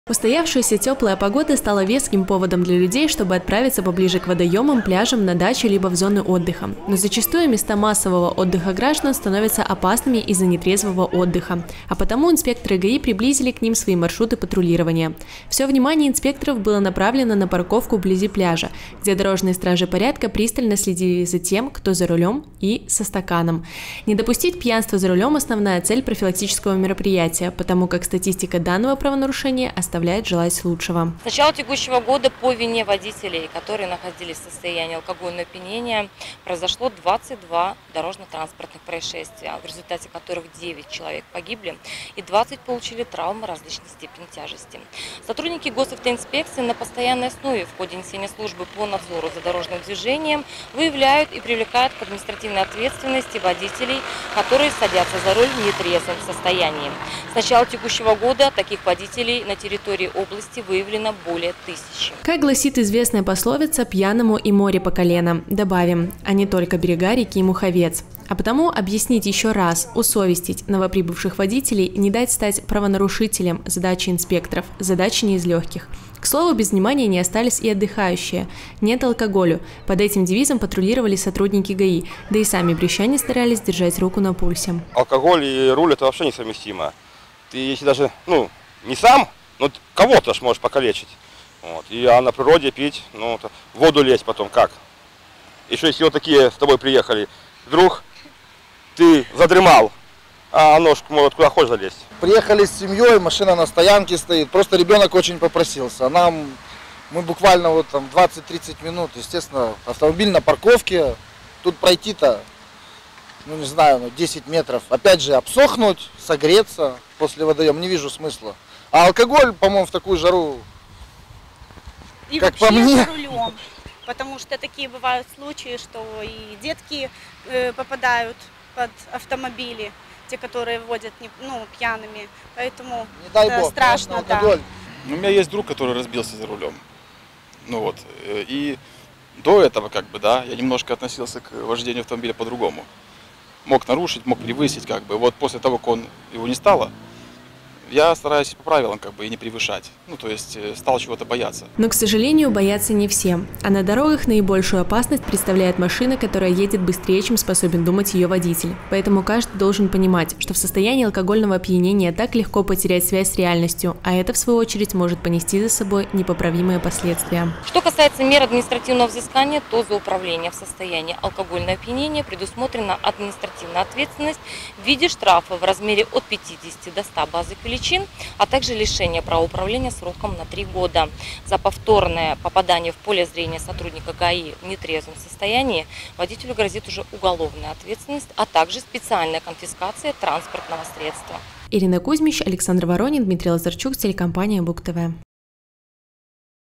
2020-06-17 г. Брест. Акция ГАИ «Нет - алкоголю». Новости на Буг-ТВ.
В жаркую погоду людей все чаще тянет на природу, к водоемам и пляжам. Зачастую не обходится без спиртного, некоторые просто не умеют отдыхать без него. К сожалению, после такого отдыха эти граждане, проявляя полную беспечность, садятся за руль и везут «под градусом» пассажиров. Для того, чтобы выявить нетрезвых водителей и отстранить их от управления, а также провести профилактические беседы с другими владельцами транспортных средств, сотрудники ГАИ организуют наряды вблизи мест массового отдыха граждан. Подробнее о профилактическом рейде, мы расскажем прямо сейчас.